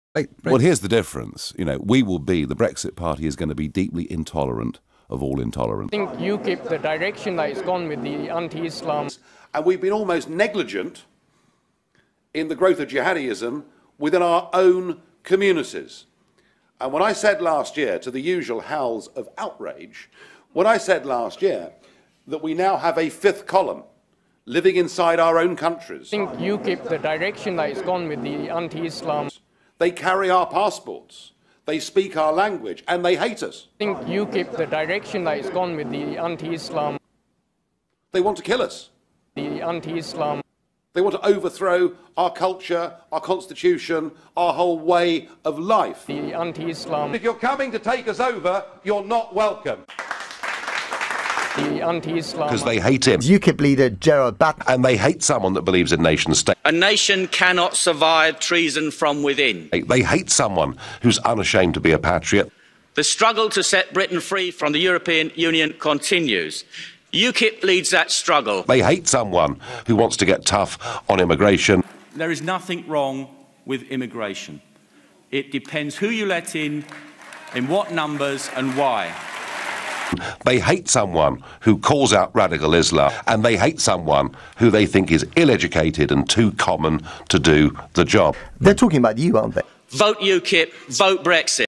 They say a week is a long time in politics
Here is a timely sense of perspective on Nigel Farage, UKIP and Gerard Batten.